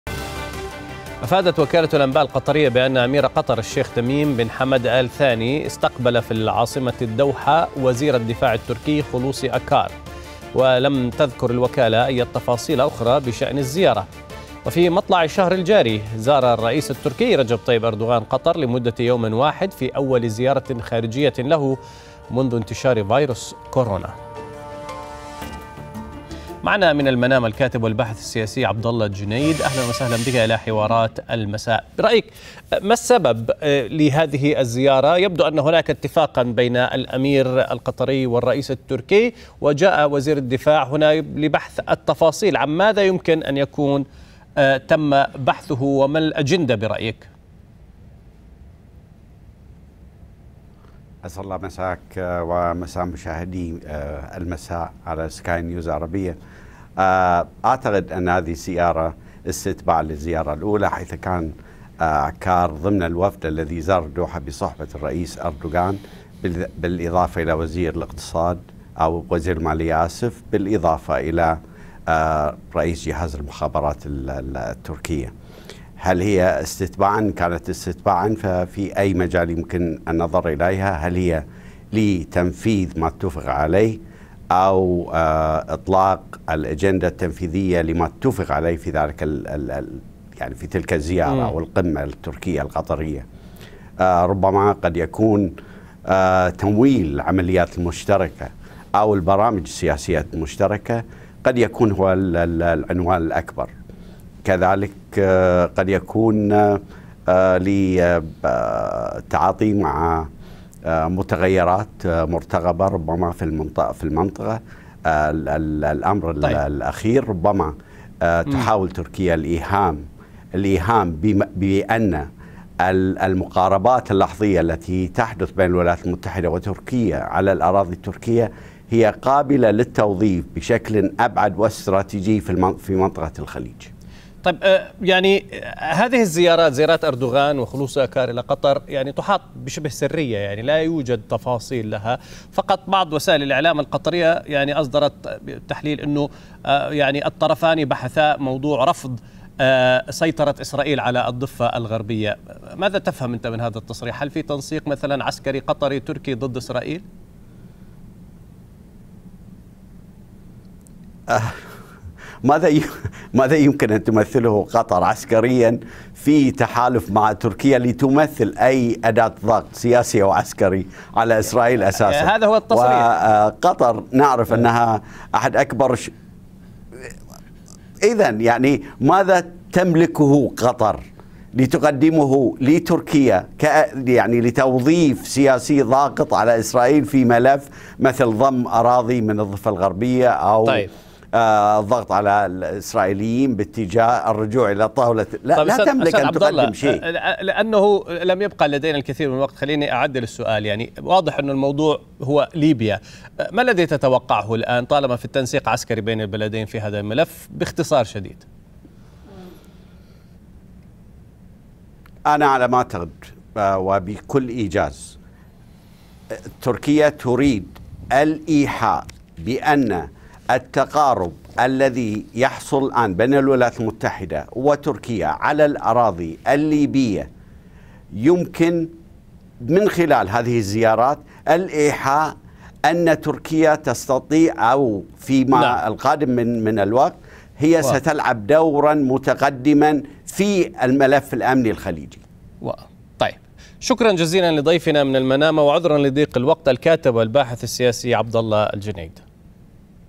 وزير الدفاع التركي يزور قطر دون ذكر أي تفاصيل عن الزيارة
أفادت وكالة الأنباء القطرية بأن أمير قطر الشيخ تميم بن حمد آل ثاني
 استقبل في العاصمة الدوحة وزير الدفاع التركي خلوصي أكار. ولم تذكر الوكالة أي تفاصيل أخرى بشأن الزيارة.
وفي مطلع الشهر الجاري زار الرئيس التركي، رجب طيب أردوغان، قطر لمدة يوم واحد في أول زيارة خارجية له منذ انتشار فيروس كورونا .